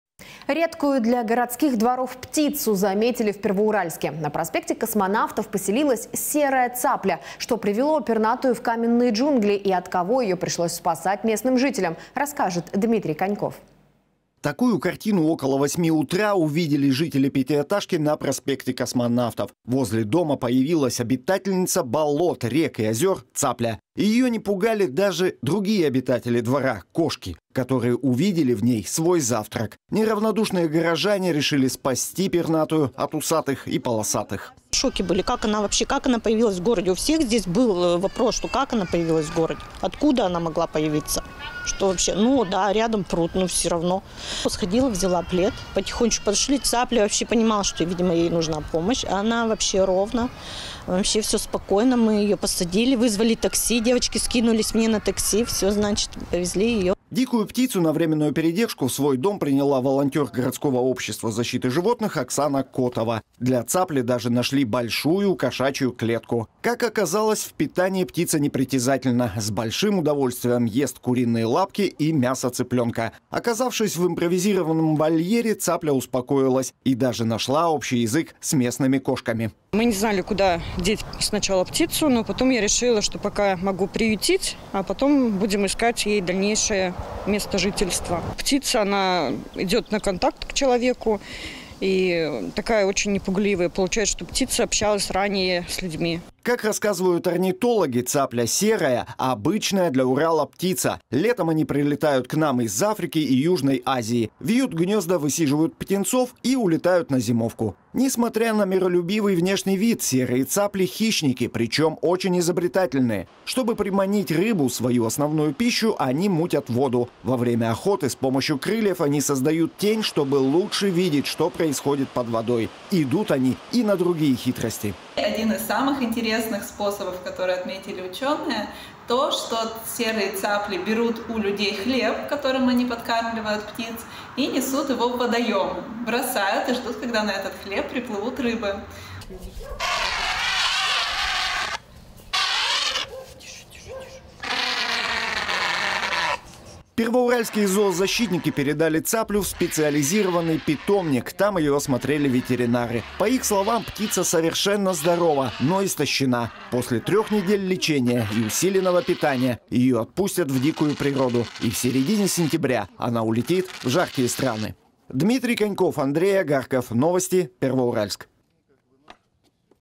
В Первоуральске нашли серую цаплю
Редкую для городских дворов птицу заметили в Первоуральске. На проспекте Космонавтов поселилась серая цапля. Что привело пернатую в каменные джунгли и от кого её пришлось спасать местным жителям расскажет Дмитрий Коньков.
Такую картину около 8 утра увидели жители пятиэтажки на пр. Космонавтов. Возле дома появилась цапля. Её не пугали даже другие обитатели двора – кошки, которые увидели в ней свой завтрак. Неравнодушные горожане решили спасти пернатую.
ЕЛИЗАВЕТА ИВАНОВА, ЖИТЕЛЬНИЦА ПЕРВОУРАЛЬСКА: "В шоке были. Как она вообще появилась в городе? Откуда? Рядом пруд, но все равно. Сходила, взяла плед. Потихонечку подошли. Цапля понимала, что ей нужна помощь. Мы её посадили в такси и повезли".
Дикую птицу на временную передержку в свой дом приняла волонтёр городского Общества защиты животных Оксана Котова. Для цапли даже нашли большую кошачью клетку. Как оказалось, в питании птица непритязательна. С удовольствием ест куриные лапки и мясо цыплёнка. Оказавшись в импровизированном вольере, цапля успокоилась и даже нашла общий язык с местными кошками.
ОКСАНА КОТОВА, ВОЛОНТЁР ОБЩЕСТВА ЗАЩИТЫ ЖИВОТНЫХ: "Мы не знали, куда сначала деть птицу. Но потом я решила приютить и искать ей дальнейшее место жительства. Птица идёт на контакт к человеку, не пугливая".
Как рассказывают орнитологи, цапля серая - обычная для Урала птица. Летом они прилетают к нам из Африки и Южной Азии, вьют гнезда, высиживают птенцов и улетают на зимовку. Несмотря на миролюбивый внешний вид, серые цапли – хищники, причём очень изобретательные. Чтобы приманить рыбу, свою основную пищу, они мутят воду. Во время охоты с помощью крыльев они создают тень, чтобы лучше видеть, что происходит под водой. Идут они и на другие хитрости.
АННА ПОСТОВАЛОВА, ЗАВЕДУЮЩАЯ НАУЧНО-ПРОСВЕТИТЕЛЬСКИМ ОТДЕЛОМ ЕКАТЕРИНБУРГСКОГО ЗООПАРКА: "Один из самых интересных способов, который отметили учёные. Они берут у людей хлеб, которым их прикармливают, несут его в водоём. Бросают и ждут, когда на этот хлеб приплывут рыбы".
Первоуральские зоозащитники передали цаплю в специализированный питомник. Там её осмотрели ветеринары. По их словам, птица совершенно здорова, но истощена. После трех недель лечения и усиленного питания её отпустят в дикую природу и в середине сентября она улетит в жаркие страны.